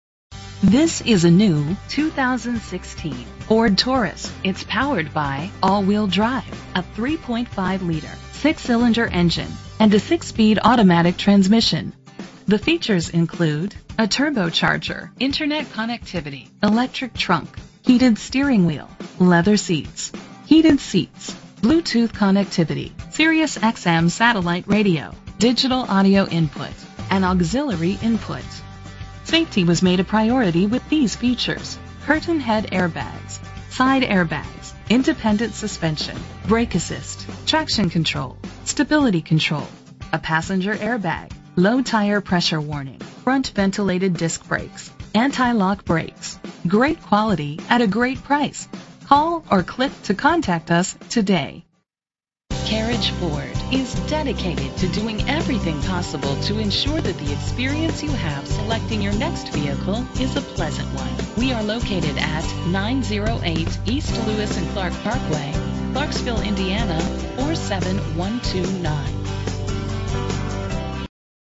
2016 Ford Taurus - Clarksville IN
Phone:
Year: 2016
Make: Ford
Model: Taurus
Trim: SHO
Engine: 3.5 liter 6 cylinder 24 valve
Transmission: 6-Speed Automatic
Color: Ruby Red Metallic Tinted Clearcoat
Mileage: 5
Address:
908 E. Highway 131